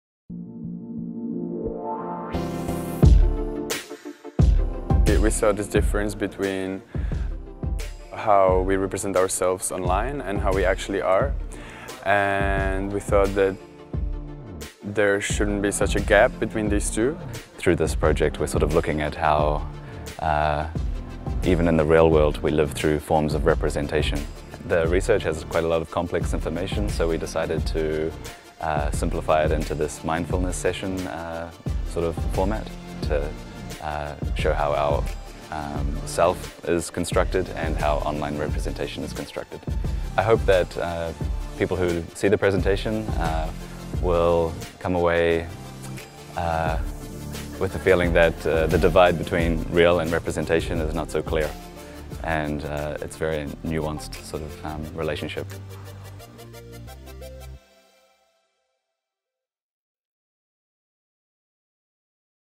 Vincent & Filip - Salone del Mobile Milano - Design Academy
Edited by myself and video shot by Mark Kwikkers  - on location throughout the week at the Milan Design Week (Salone del Mobile Milano).